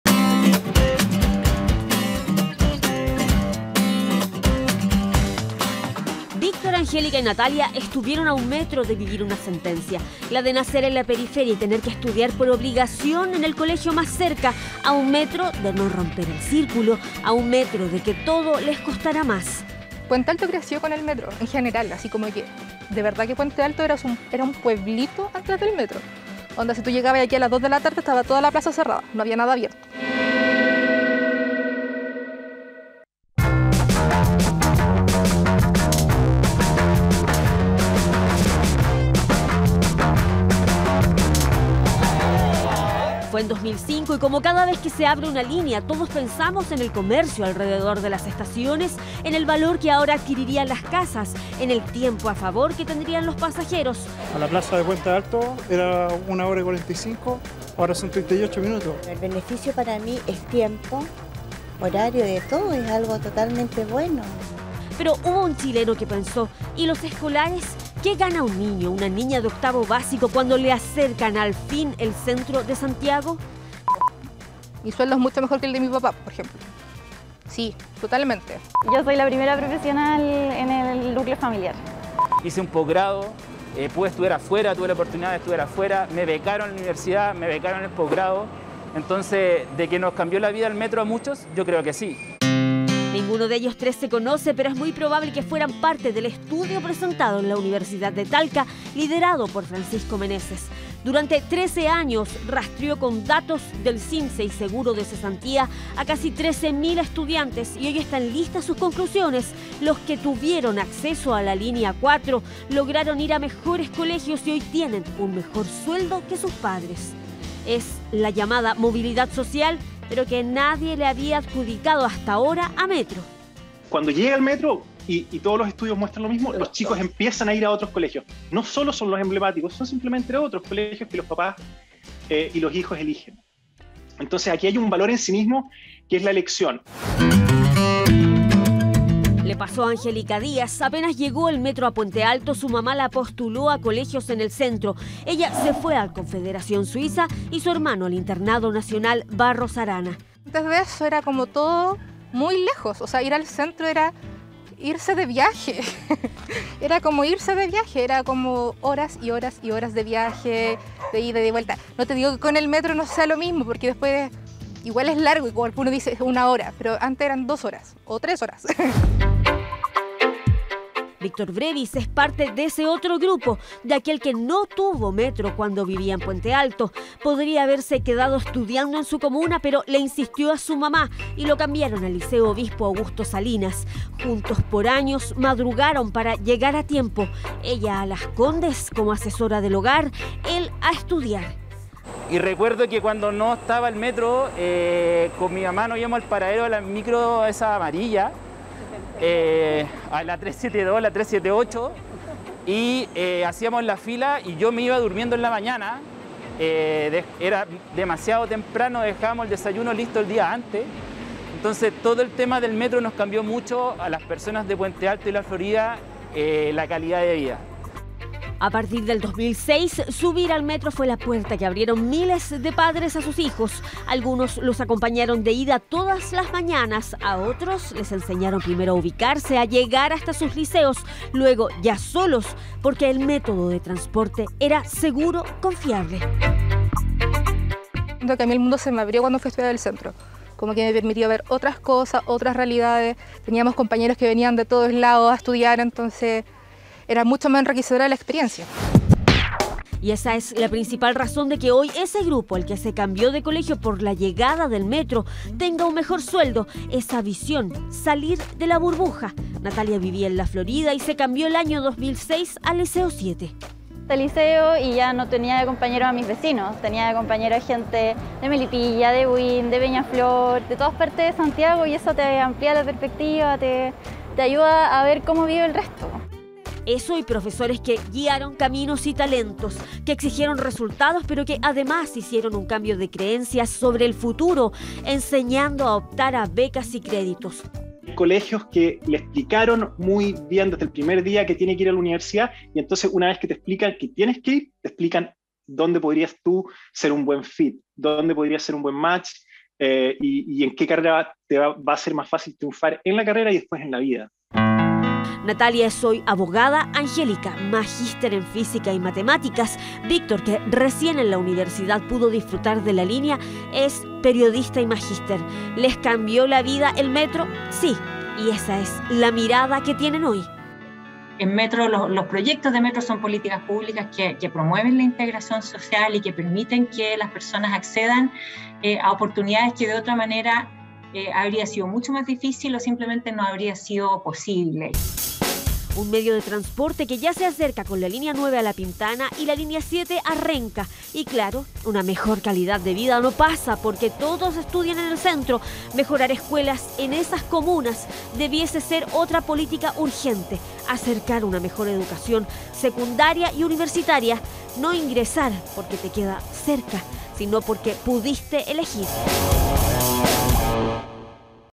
Linea 4: estudiantes "a un metro" de mejores oportunidades
Cada vez que se construye una nueva línea del Metro sabemos que traerá beneficios para el comercio, la plusvalía del sector y un ahorro en tiempos de viaje. Pero hay un beneficio q hasta ahora no se había medido. Un estudio revela que estudiantes de La Florida y Puente Alto ganan mejores sueldos que sus propios padres solo porque accedieron a mejores colegios gracias a la la Línea 4 del subterráneo.